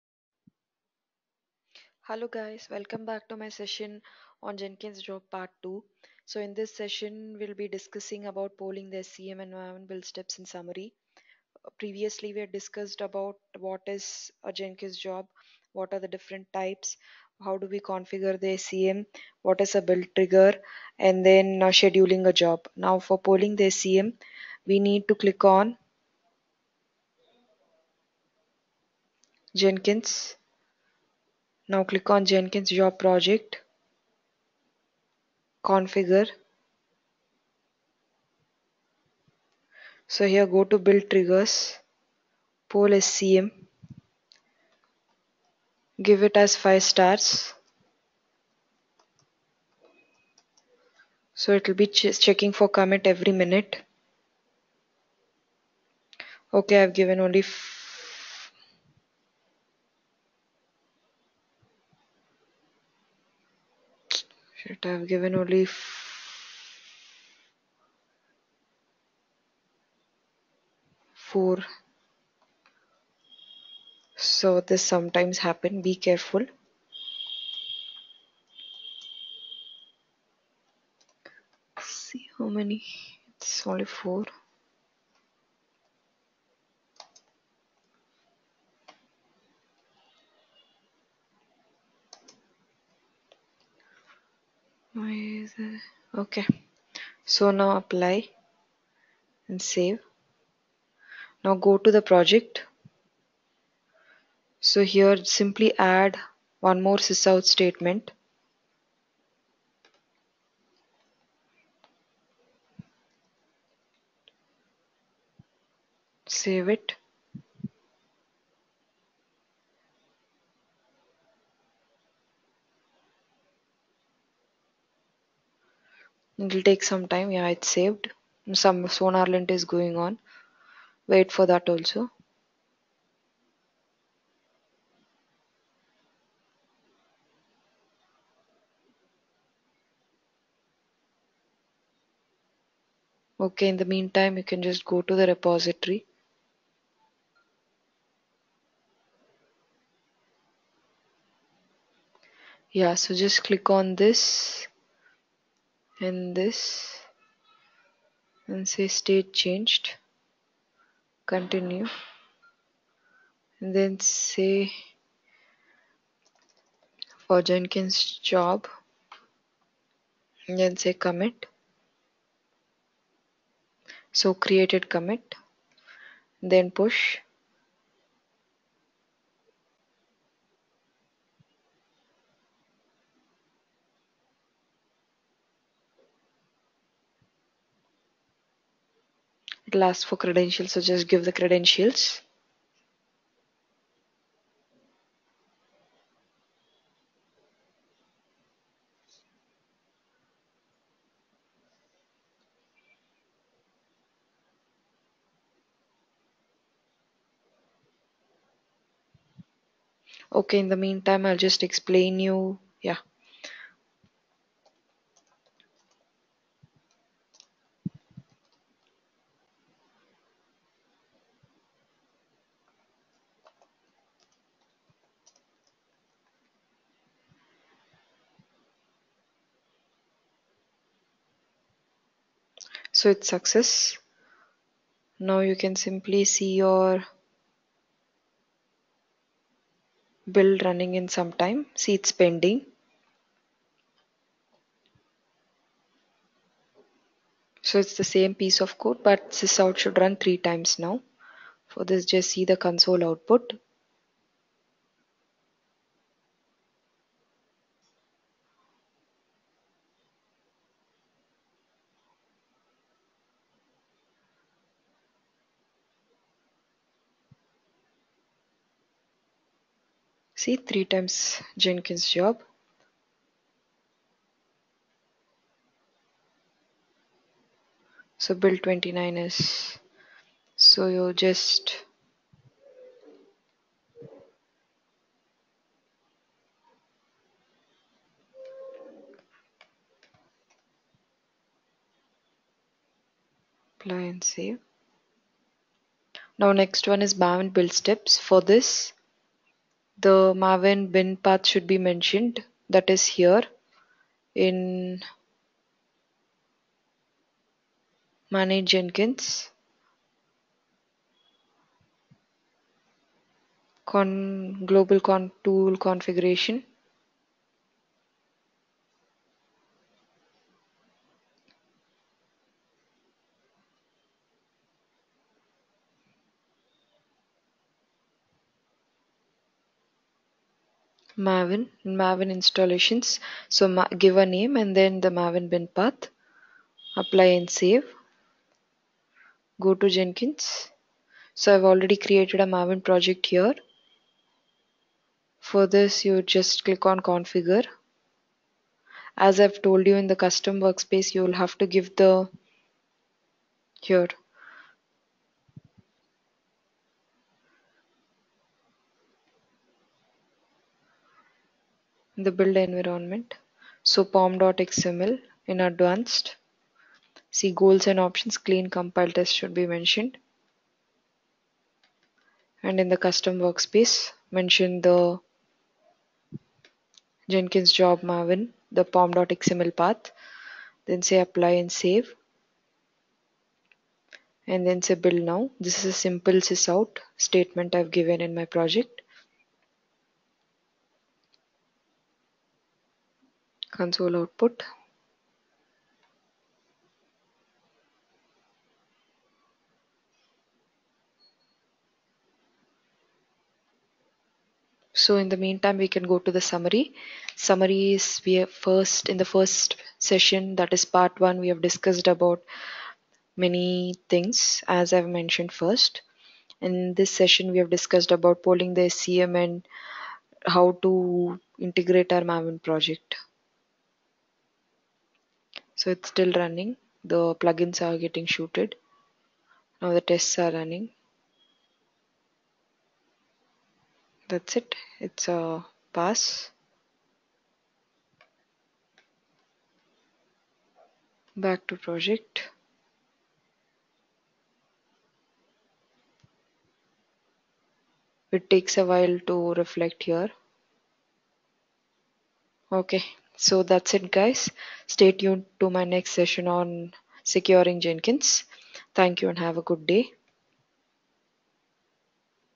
Jenkins Job: Configuring SCM, Build Jobs And Polling The SCM: part 2
In this tutorial, we will be discussing everything about a Jenkins job, Polling the SCM, Maven Build Steps